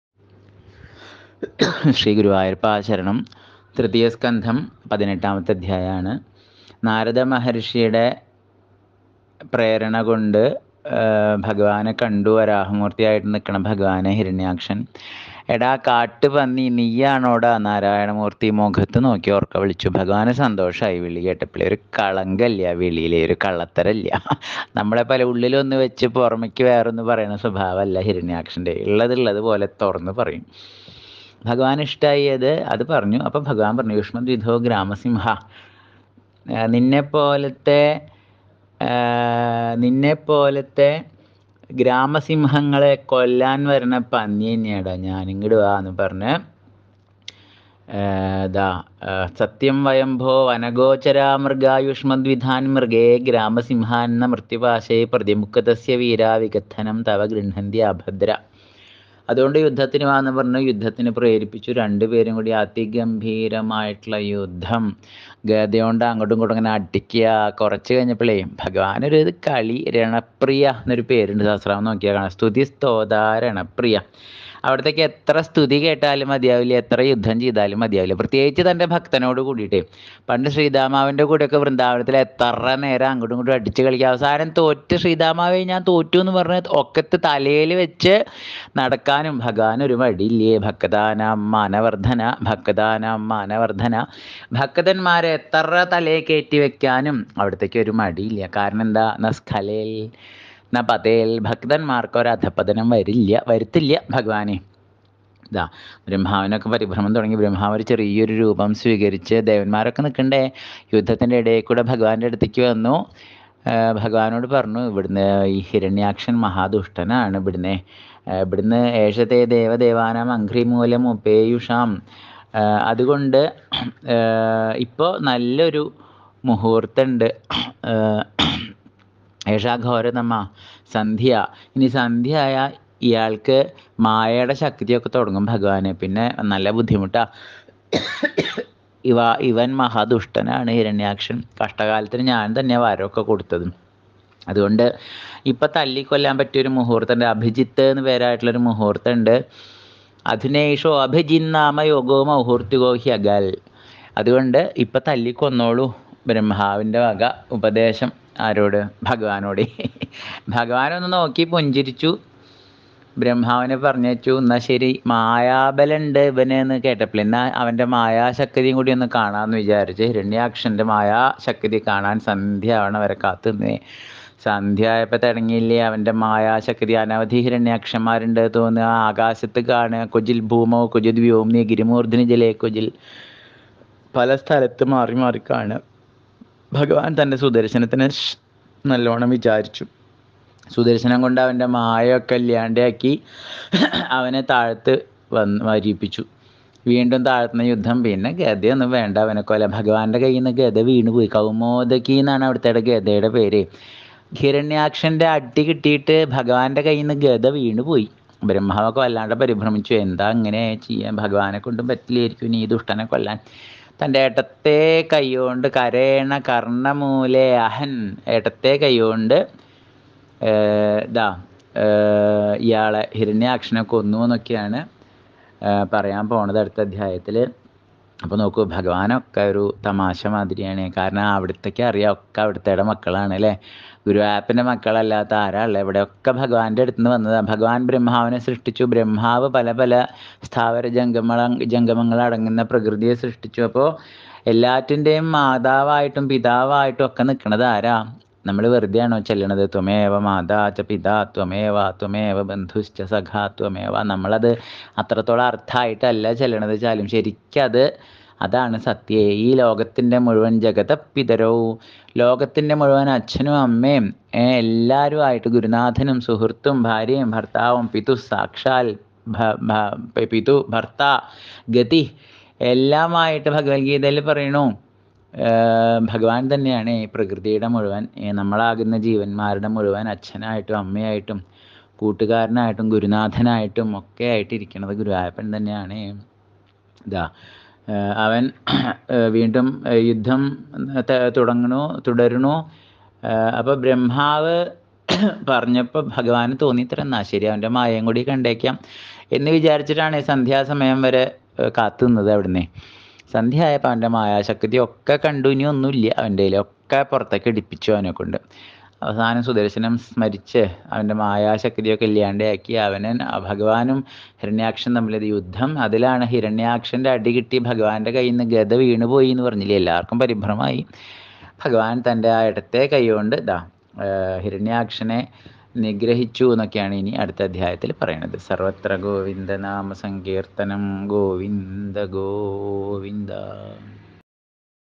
ശ്രീമദ് ഭാഗവതം തൃതീയ സ്കന്ധം അദ്ധ്യായം 18 ഗുരുവായൂർ ഹരി നമ്പൂതിരി
ഭാഗവത കഥകൾ കേൾക്കു അതിലൂടെ ഭഗവാന്റെ അടുത്തേക്ക് നടന്നു നീങ്ങാം ഗുരുവായൂർ ഹരി നമ്പൂതിരി guruvayoor temple bhagavatham harekrishna radhe unnikannan god devotional stories